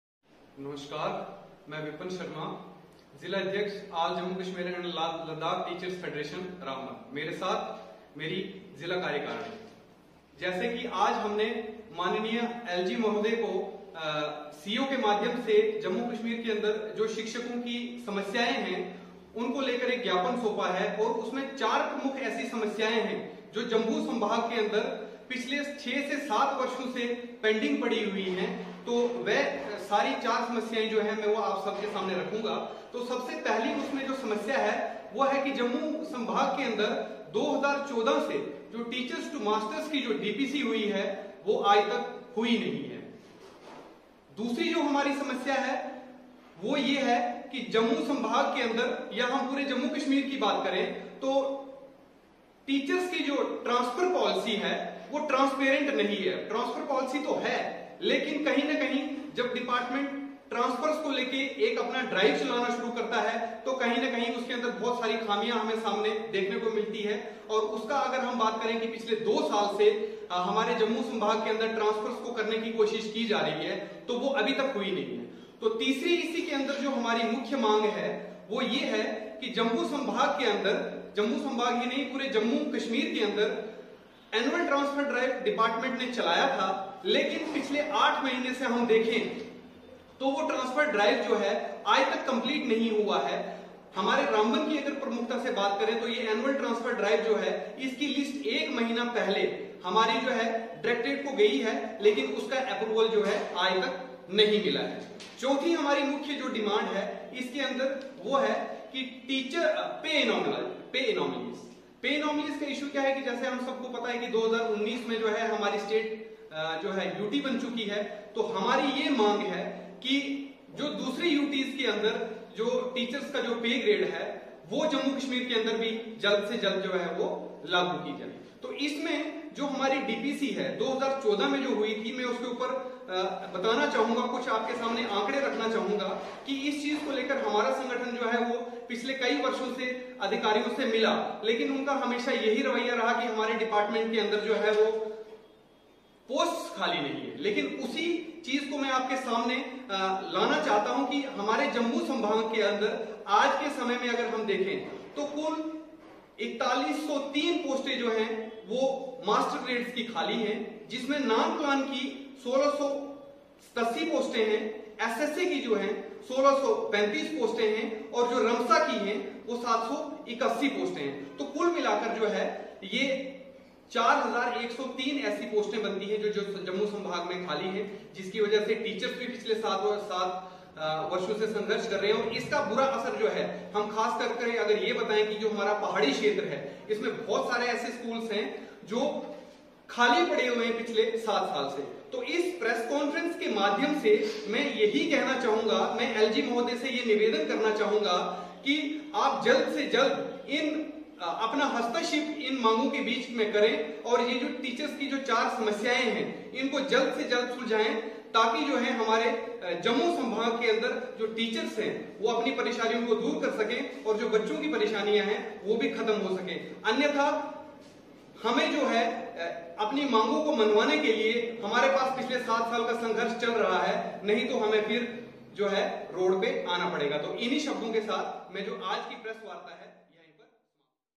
ALL JAMMU KASHMIR & LADAKH TEACHER FEDERATION HIGHLIGHT TEACHERS' DEMANDS
All Jammu Kashmir and Ladakh Teachers Federation, District Ramban held a press conference in which Vipan Sharma, President AJKLTF Ramban highlighted the pending issues of Teaching Faculties like Teachers-Masters DPC, Transparent Transfer Policy, Annual Transfer Drive and Pay Anomaly etc.